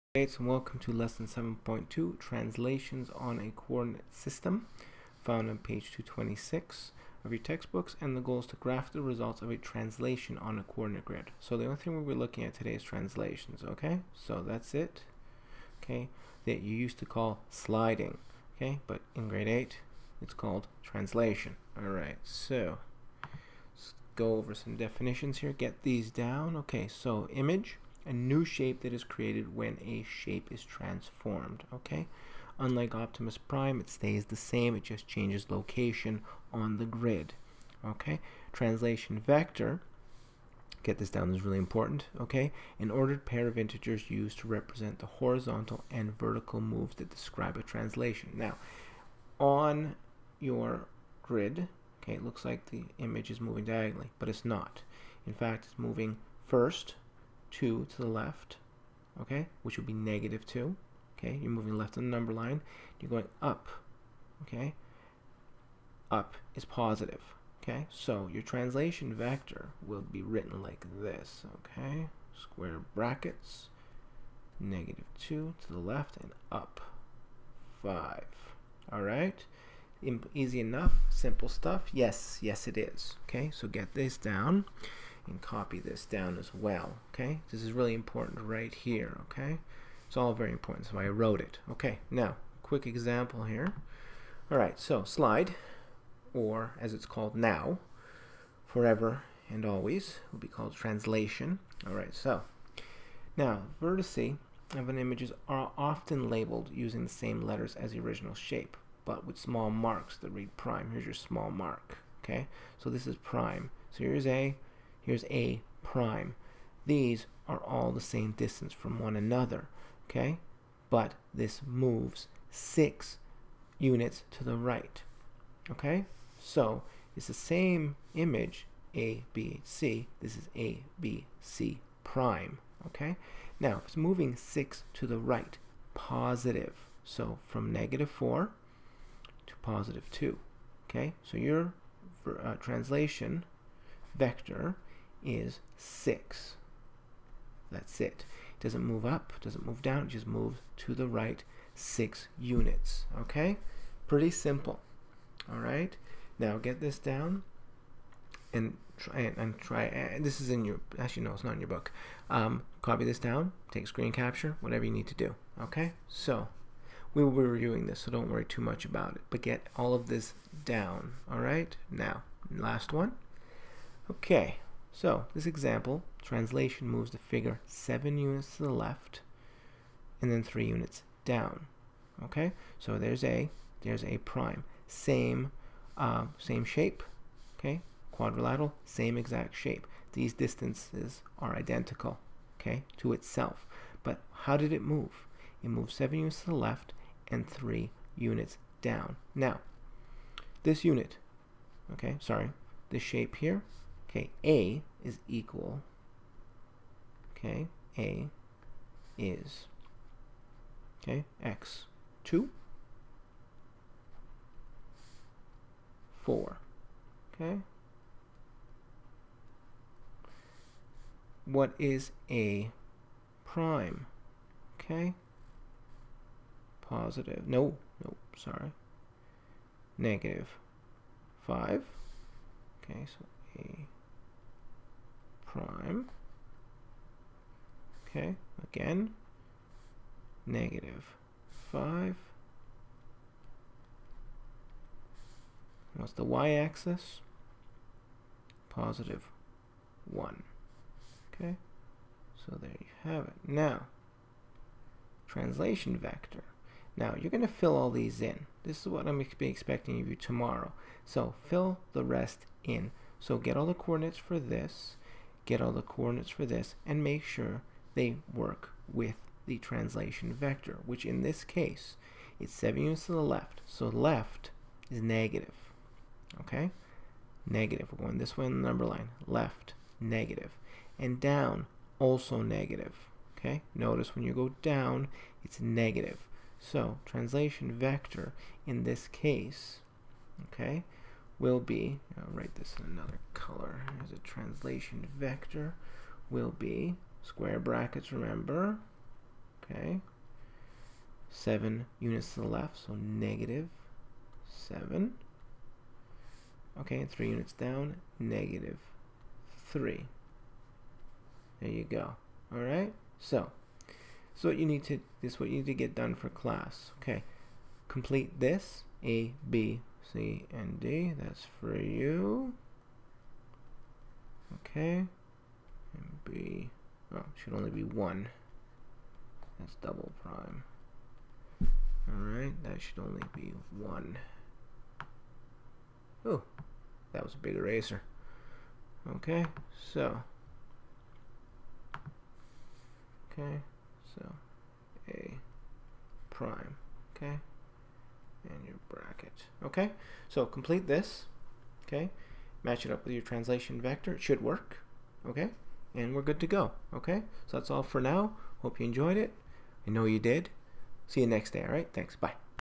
Lesson 7.2: Translations on a Coordinate System
GOAL
Graph the results of a translation on a coordinate grid.